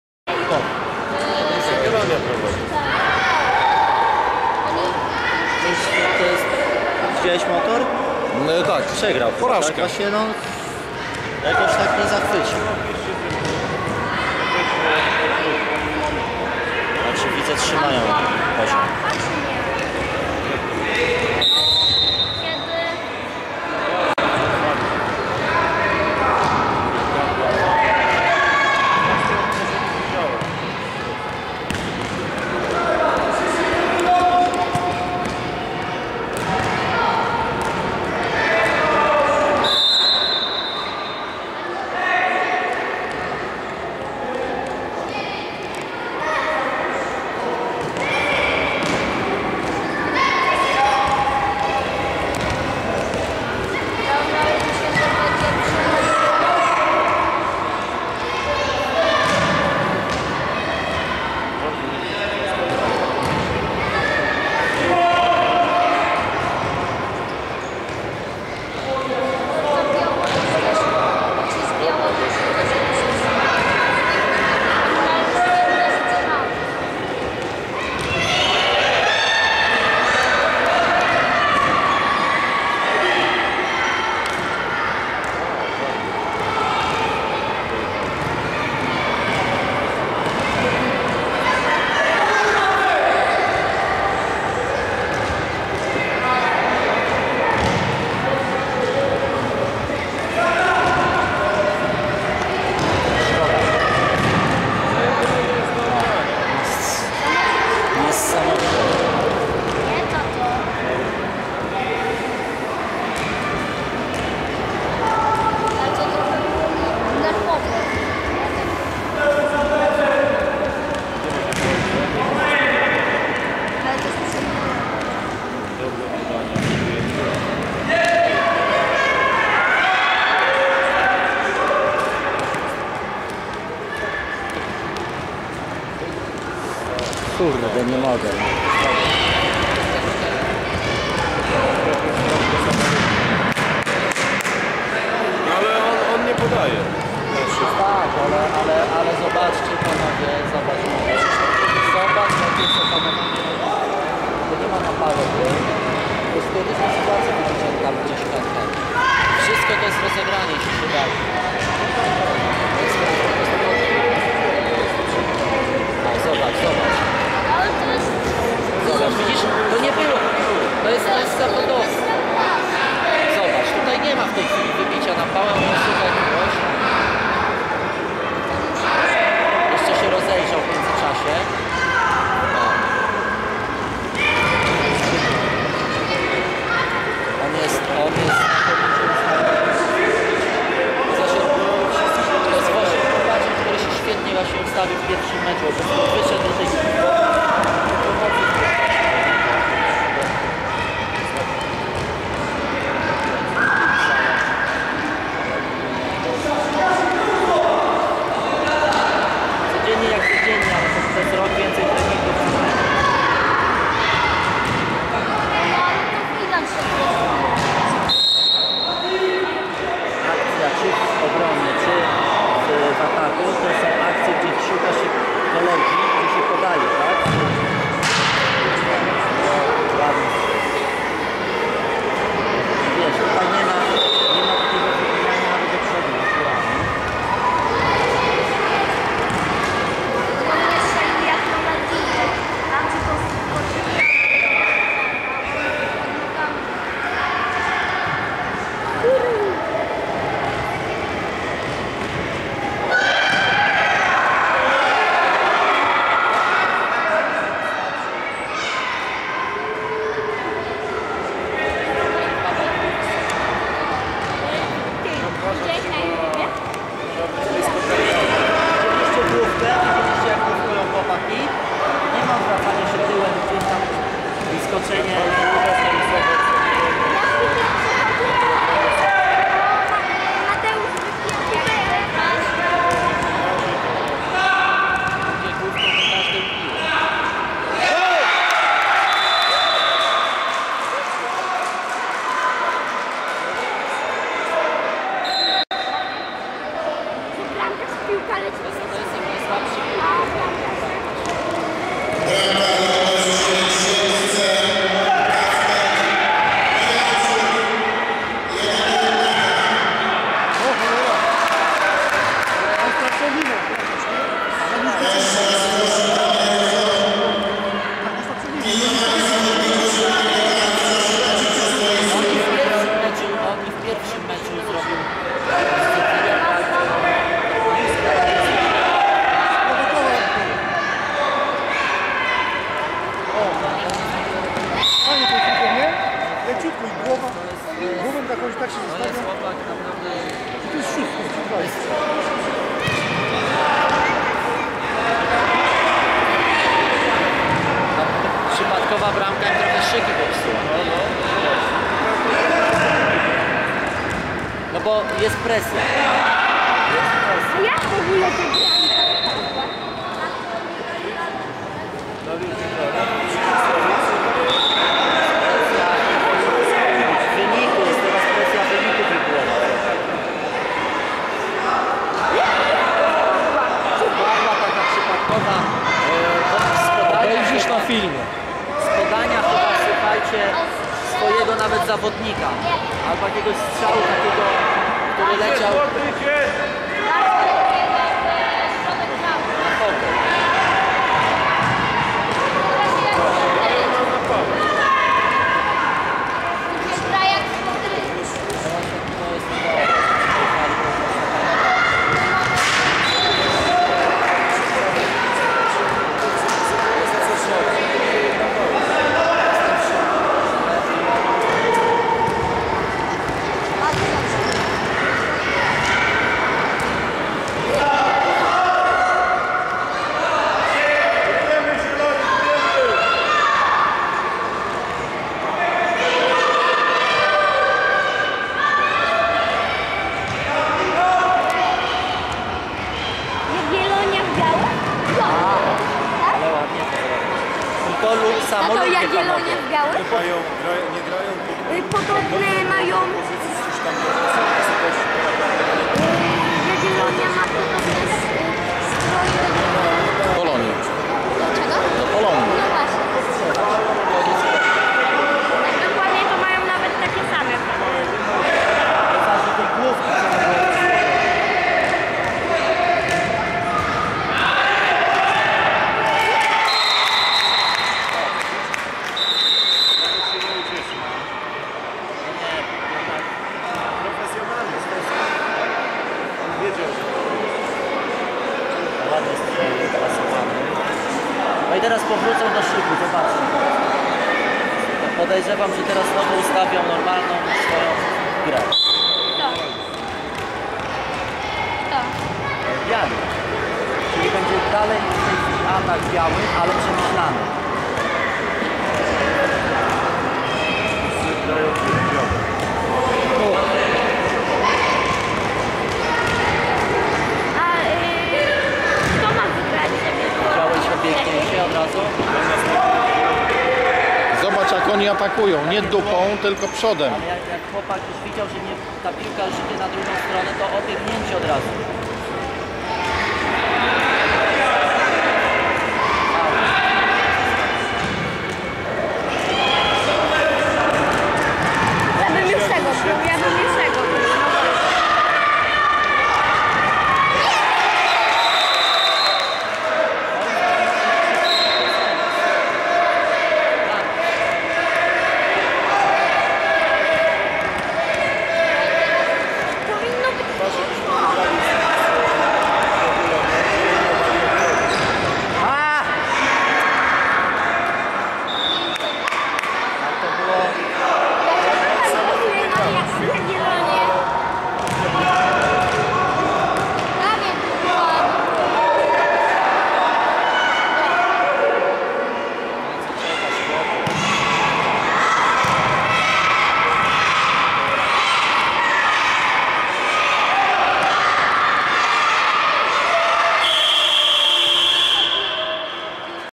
SANEA CUP. Grupa B - UKS 9 Siedlce vs. Jagiellonia Białystok.wmv
SANEA CUP - Turniej piłkarski rocznika 2000. Biała Podlaska, 9 stycznia 2010r.
Faza grupowa.